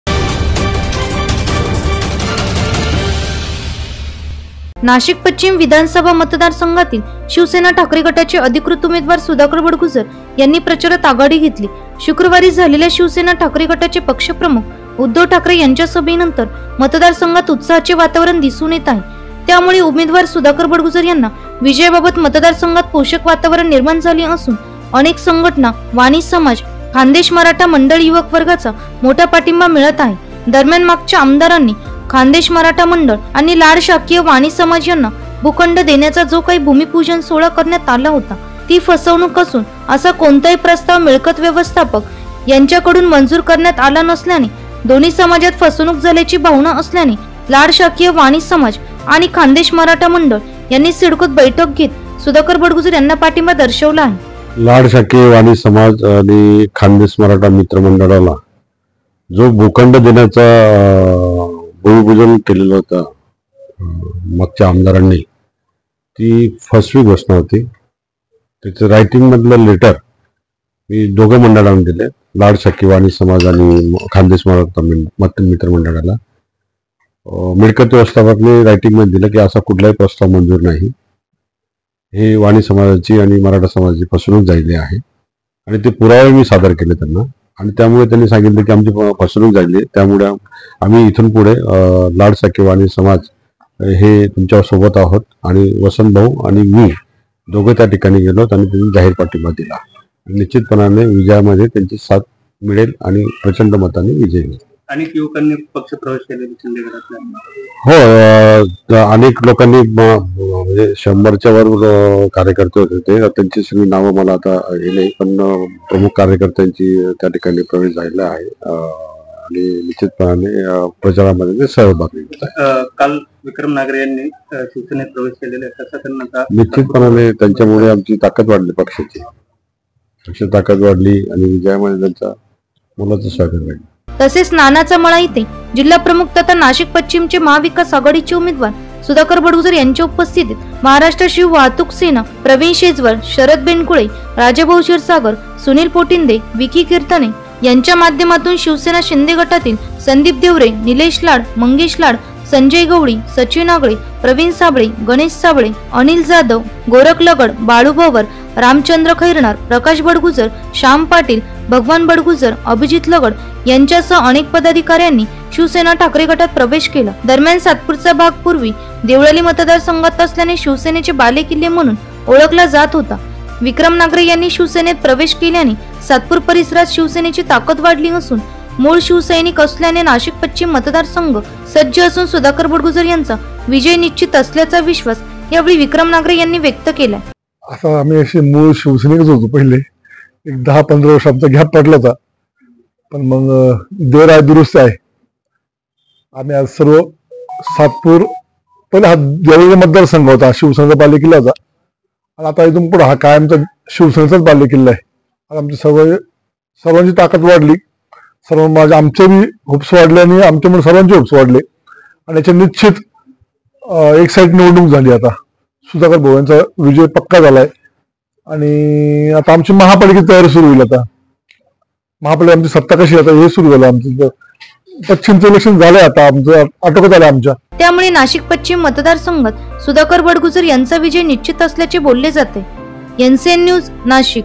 नाशिक । खान्देश मराठा मंडळ आणि लाड शाखीय वाणी समाज यांचा सुधाकर बडगुजर यांना पाठिंबा...
NCN News